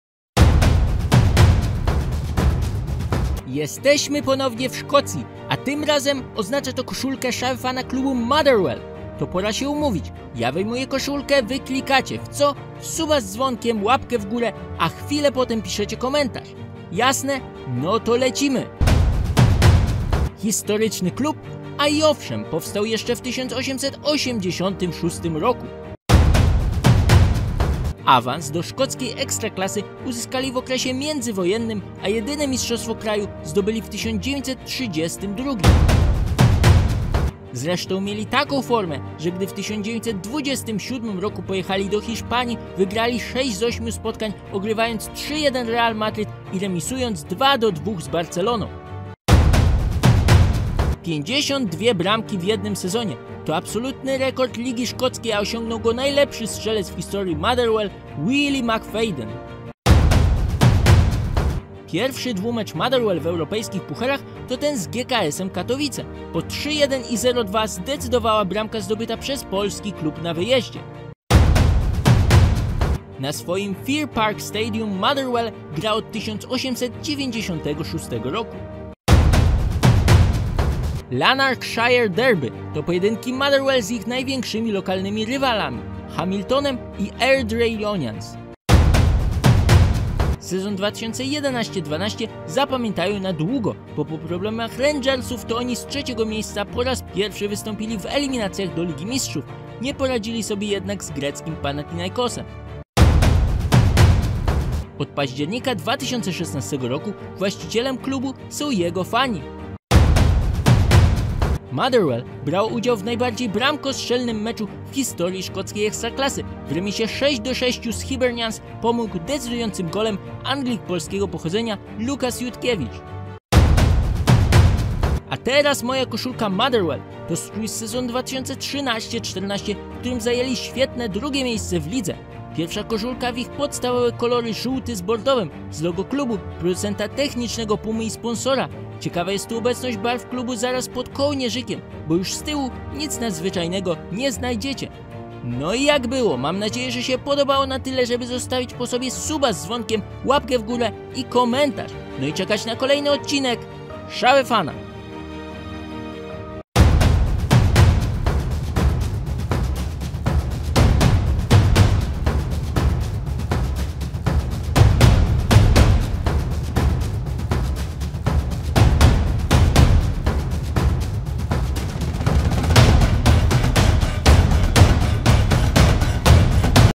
Motherwell ciekawostki i koszulka - Szafa Fana 116
Szafa Fana znów w Szkocji. Pora więc na Motherwell ciekawostki i koszulkę z mojej kolekcji. Zachęcam do oglądania do końca i klikania łapek w górę i subów z dzwonkami Wbijajcie też po więcej na Insta